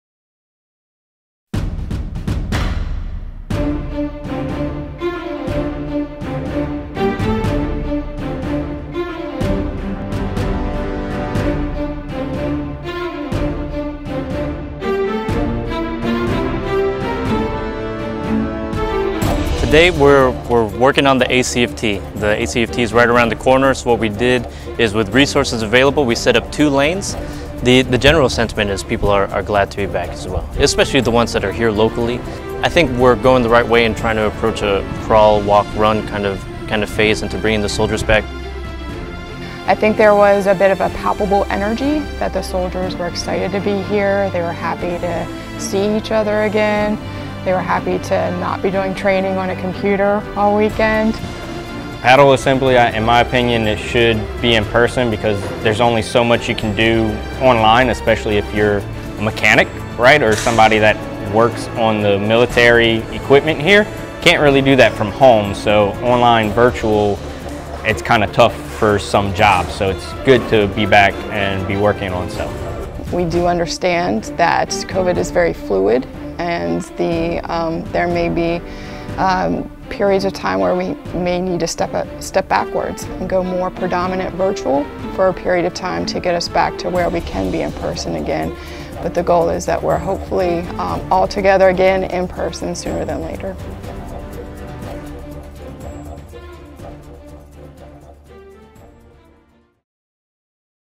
Getting back to basics
A special thank you to all of the Army Reserve Soldiers of the 377th Theater
Sustainment Command who assembled for the first in-person battle assembly since the pandemic began. Find out what it was like getting back into the swing of things as Soldiers local to the greater New Orleans area reported in to the headquarters building and got the job done over the drill weekend.